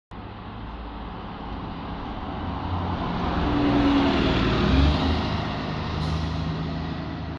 Short Observation | T244 seen approaching Mandeville Road heading to Waltham Cross
Yes I've broken the every 4 day upload but since I always upload late, I'm gonna upload a filler